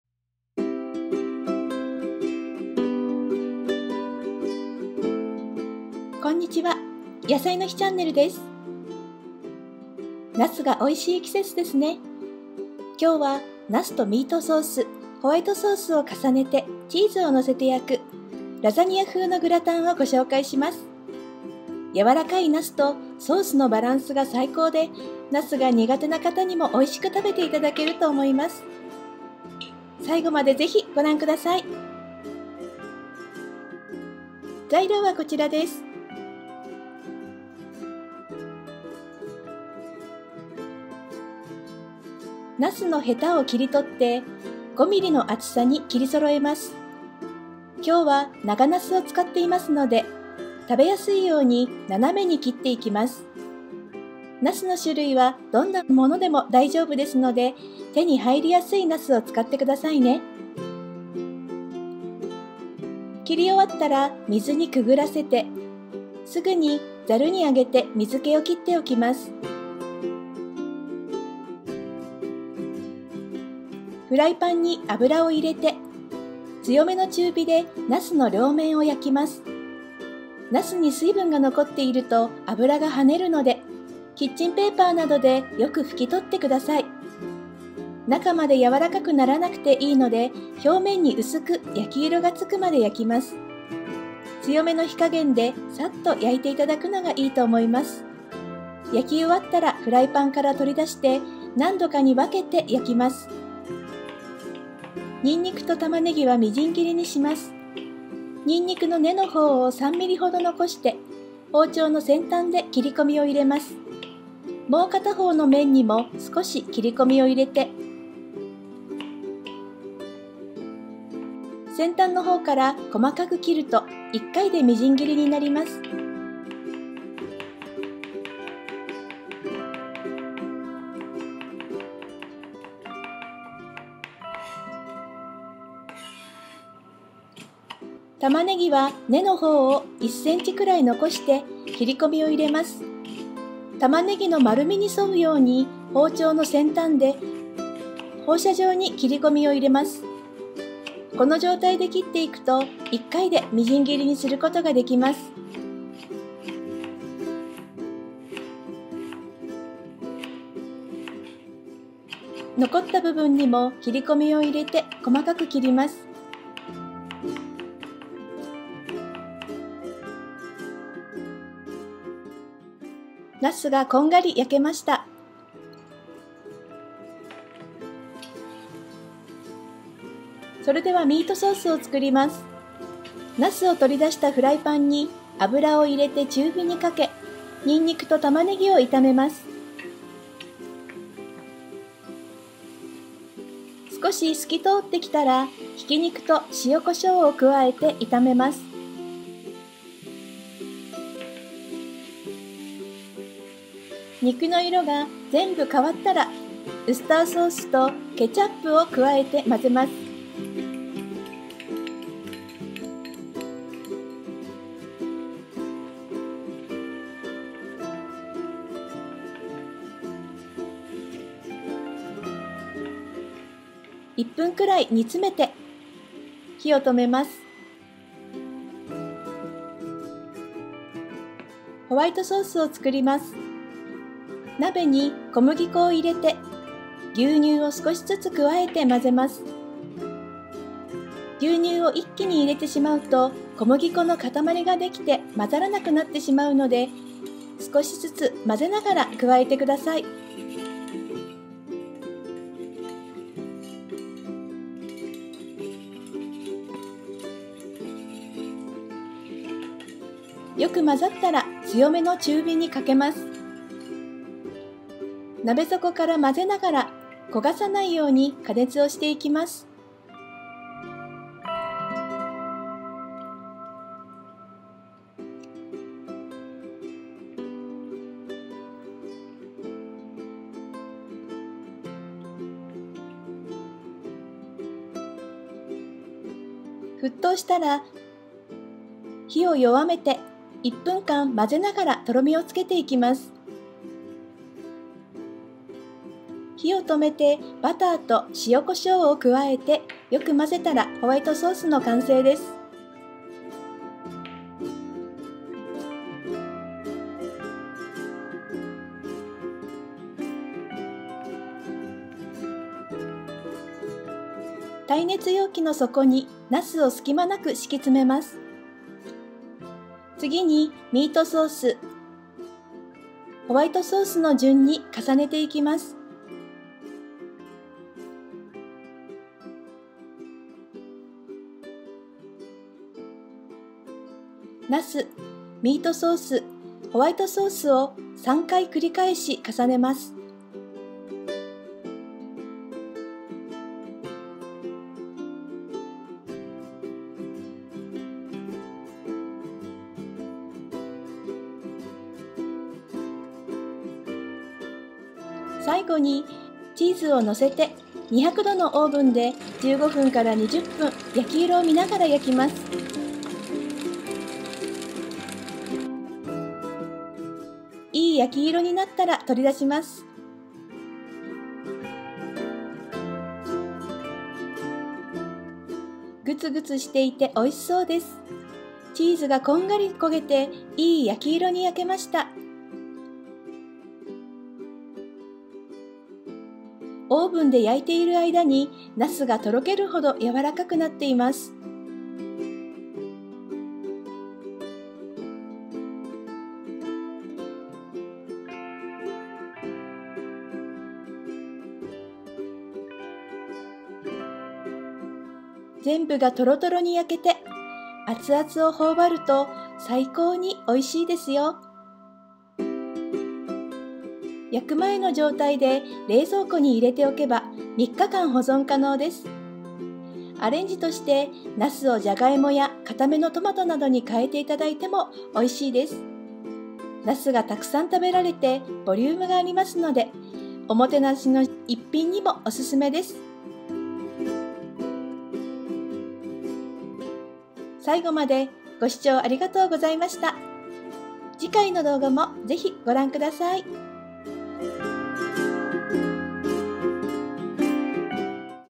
（作り置き料理）ナスの大量消費に！「ナスのミルフィーユグラタン」Eggplant Millefeuille Gratin（English subtitle)
ナス３００ｇ・合いびき肉２００ｇ・ニンニク１片・タマネギ1/4個・食用油大さじ２〜３・ウスターソース大さじ２・ケチャップ大さじ１・ピザ用チーズ８０ｇ
（ホワイトソース材料）牛乳３００ｇ・小麦粉大さじ３・有塩バター１０ｇ・コショウ少々
300 g eggplant, 200 g ground meat, 1 piece garlic, 1/4 onion, 2 tablespoons cooking oil, 2 tablespoons Worcester sauce, 1 tablespoon ketchup, 80 g cheese for pizza
(White sauce material) 300g milk, 3 tablespoons flour, 10g salted butter, a little pepper

ナスが美味しい季節ですね。
今日は、ナスとミートソース、ホワイトソースを重ねてチーズをのせて焼く、ラザニア風のグラタンをご紹介します。
柔らかいナスとソースのバランスが最高で、ナスが苦手な方にも美味しく食べていただけると思います。
ナス、ミートソース、ホワイトソースを３回繰り返し重ねます。
最後にチーズをのせて２００度のオーブンで１５分〜２０分焼き色を見ながら焼きます。
オーブンで焼いている間に、ナスがとろけるほど柔らかくなっています。
全部がトロトロに焼けて、熱々をほうばると最高に美味しいですよ。

焼く前の状態で冷蔵庫に入れて置けば３日間保存可能です。
アレンジとして、ナスをジャガイモや硬めのトマトなどに変えていただいても美味しいです。
ナスがたくさん食べられて、ボリュームがありますので、おもてなしの１品にもおすすめです。

Today, I would like to introduce lasagna-style gratin, which is made by layering eggplant, meat sauce, and white sauce on top of cheese and baking.
The perfect balance of tender eggplant and sauce makes it a delicious dish for those who don't like eggplant.
Cut off the eggplant and cut it to a thickness of 5 mm.
I use long eggplant today, so I'll cut it diagonally to make it easier to eat.
Any kind of eggplant can be used, so please use an eggplant that is easy to obtain.
When you have finished cutting, pour it through water and immediately raise it in a colander to drain it.
Put oil in a frying pan and bake both sides of eggplant over medium heat.
If water remains on the eggplant, the oil will splash, so wipe it off well with kitchen paper.
It doesn't have to be soft to the inside, so bake until the surface has a light brown color.
I think it is better to bake it with a strong heat.
After baking, remove from the pan and bake in several portions.
Finely chop the garlic and onion.
Leave about 3 mm of garlic root and make a notch at the end of the knife.
Make a small incision on the other side, and cut finely from the tip to cut it into one piece.
For the onion, make a notch leaving about 1 cm of the root.
Make a thin radial cut with the tip of the knife along the onion's roundness.
If you cut it in this state, you can chop it once.
Make a small cut in the remaining part.
The eggplant was burnt.
Let's make meat sauce.
Add the oil to the frying pan from which the eggplant was taken out and heat it over medium heat to fry the garlic and onion.
When it becomes a little clear, add minced meat and salt and pepper and fry.
When all the meat color changes, add Worcester sauce and ketchup and mix. Boil for about 1 minute and turn off the heat.
Make a white sauce.
Put flour in a pan and add milk little by little and mix.
If you add milk all at once, a lump of flour will form and it will not mix, so please add it while mixing little by little.
When mixed well, heat over medium heat.
Heat from the bottom of the pot while mixing so that it does not burn.
When it boils, reduce the heat and mix for 1 minute to thicken the mixture.
Turn off the heat, add butter and salt and pepper and mix well.
Spread eggplants tightly on the bottom of the heat-resistant container.
Next, layer the meat sauce and white sauce in that order.
Repeat eggplant, meat sauce and white sauce three times.
Finally, add cheese and bake in a 400F degree oven for 15 to 20 minutes while watching the color.
When the color turns nice, remove it.
The cheese burned to a nice brown color.
While baking in the oven, the eggplants are soft enough to melt.
If you put it in the refrigerator before baking, it can be stored for 3 days.
As an arrangement, changing the eggplant to potatoes or hard tomatoes is also delicious.
You can eat a lot of eggplant and it has a lot of volume, so it's also recommended for one item of hospitality.